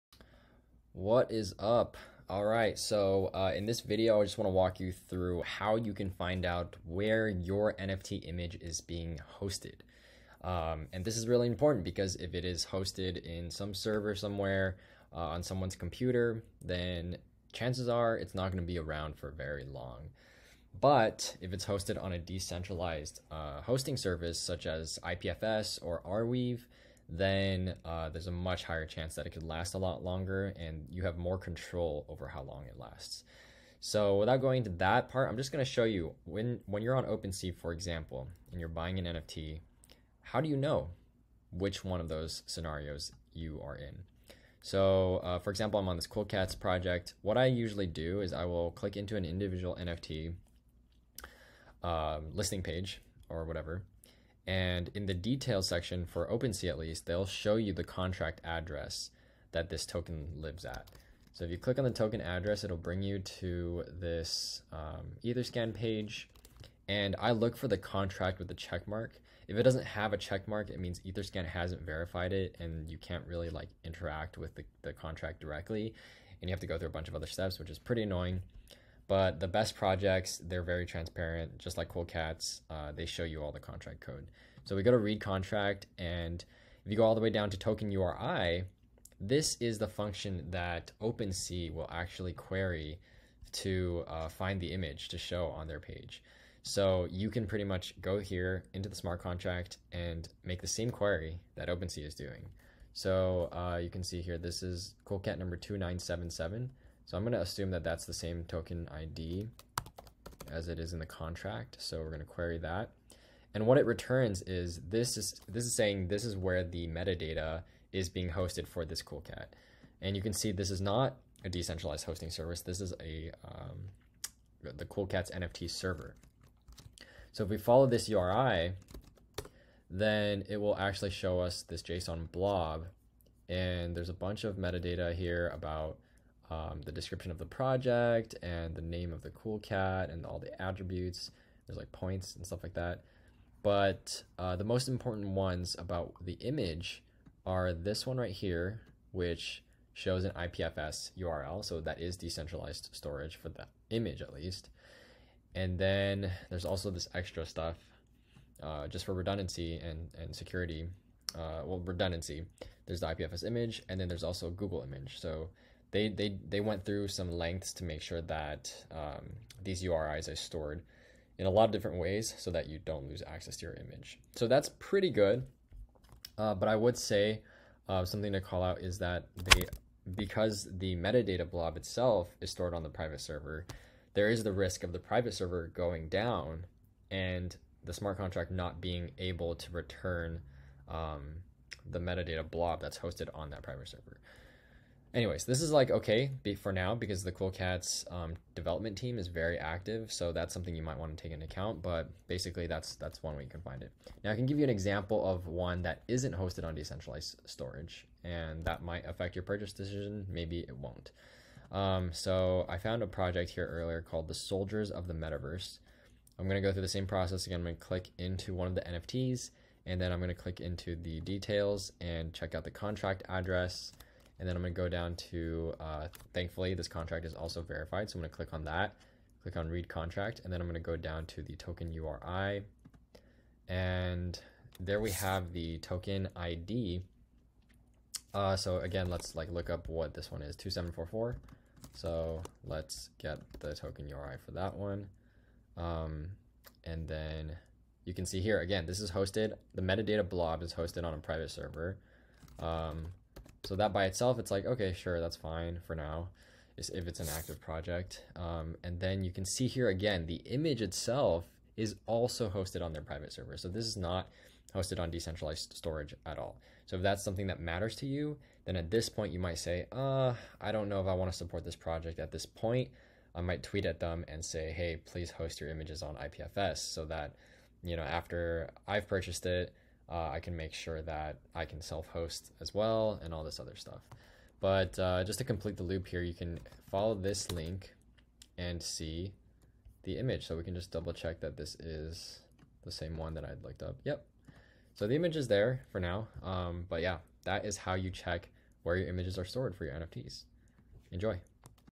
Will your NFT images last forever? (IPFS metadata)
When you're shopping for NFTs, you want to make sure that the assets you're buying have a chance of lasting a long time.

It's GOOD for projects to use decentralized services like IPFS and Arweave, because there can be redundancy across the internet for storing these images (you can even choose to host them yourself after buying!).

It's BAD for projects to host on cloud providers like AWS or Google Cloud, because once the developers stop paying for the service, your image and your metadata go "bye-bye".